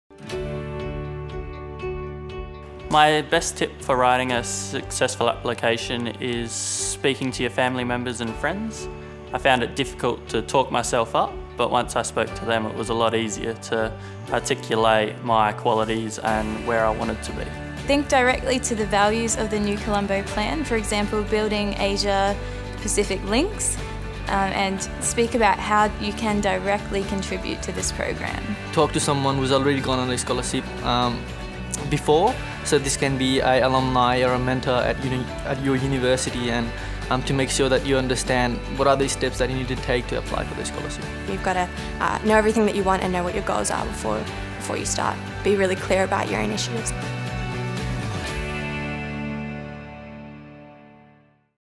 What is your best tip for writing a successful New Colombo Plan Scholarship application?
What is your best tip for writing a successful New Colombo Plan Scholarship application?  2019 Scholars answer the question.